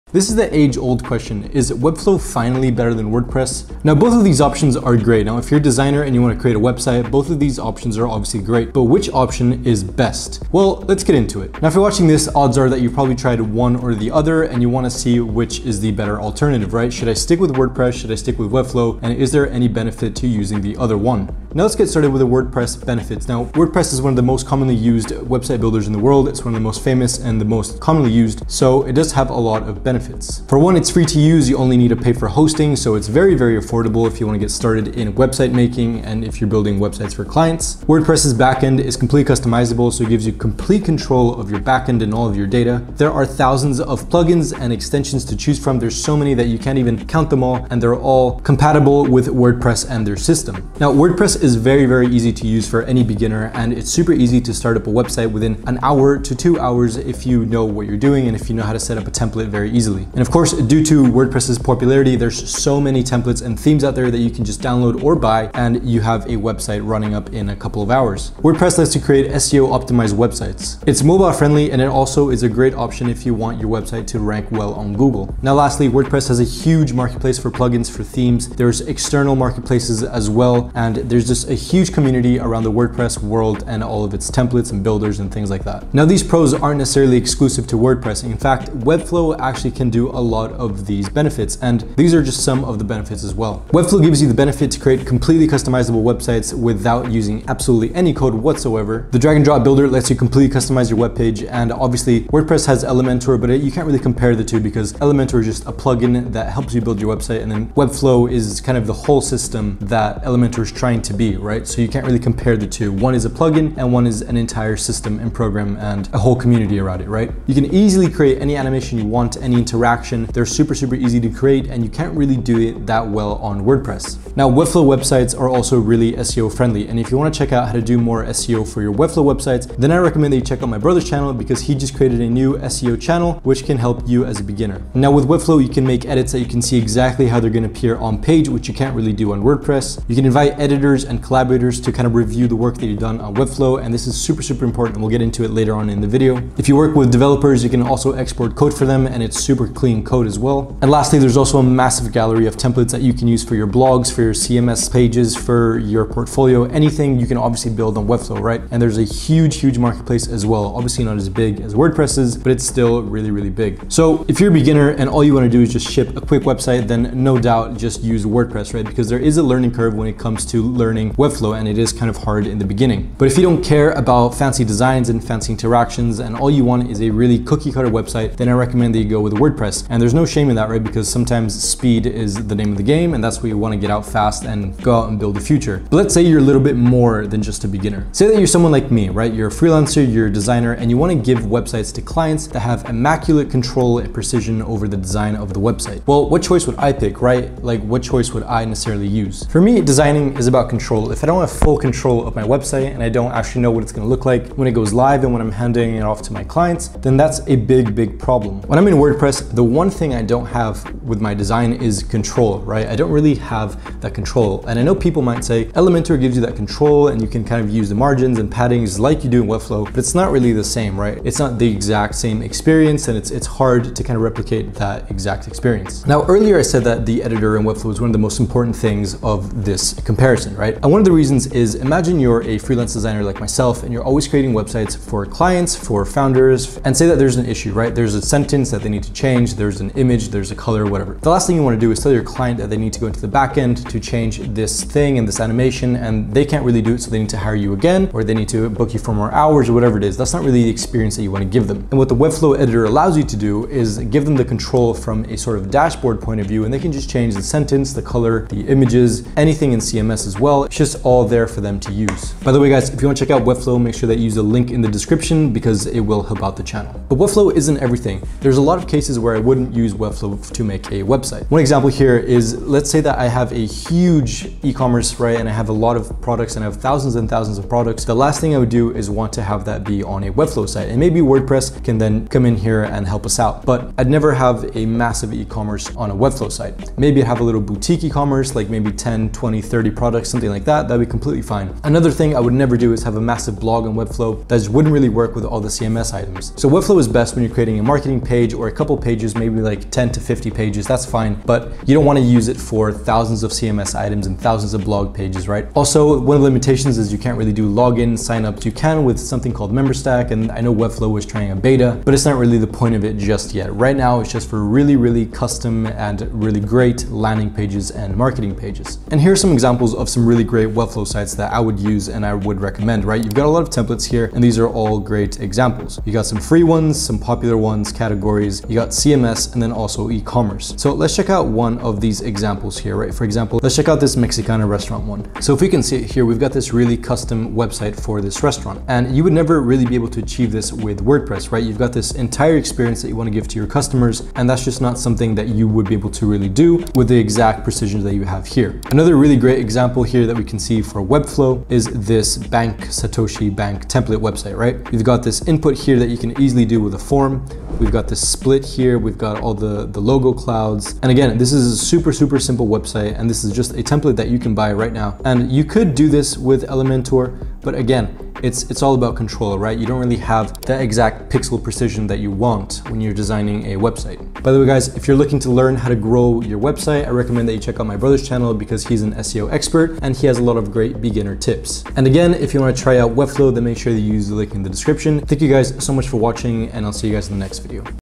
Why Webflow is Better Than Wordpress in 2021!
This is why Webflow is better than WordPress in 2021! When designing a website, it is difficult to choose which option to use, Webflow or WordPress? For designers the choice has been easy since the start, Webflow allows more customizability & control on your website, but WordPress has always been around. Lately, Webflow's capabilities are growing and it's time to revisit the age old question, is WordPress really still better than Webflow? There never will be a 100% defining factor, the choice will be different depending on your needs. But for me, Webflow is my choice for design.

👇 Try Weblflow!

0:00 Intro
0:22 WordPress benefits
1:30 Webflow benefits
2:45 If you’re a beginner which would I recommend?
3:18 What choice would an expert pick & use?
5:20 Some best Webflow examples